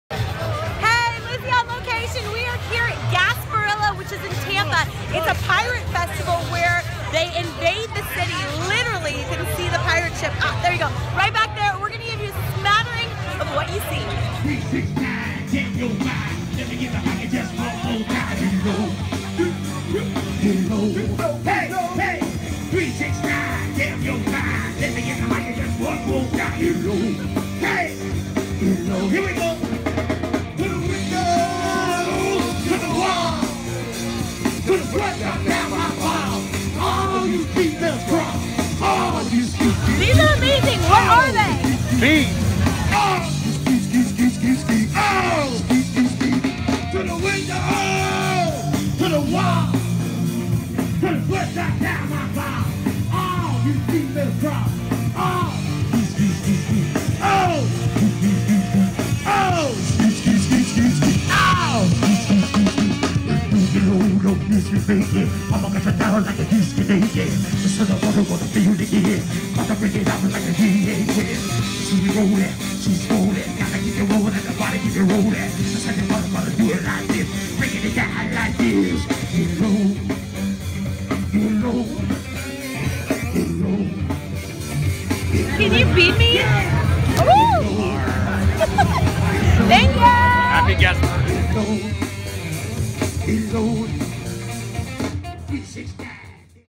Ahoy matey: Gasparilla, Tampa 2017
Pirates invade the shores of Tampa,  and there's a giant party in the streets! If mardis gras is your thing, grab ye wench, walk the plank to Tampa, and check this out!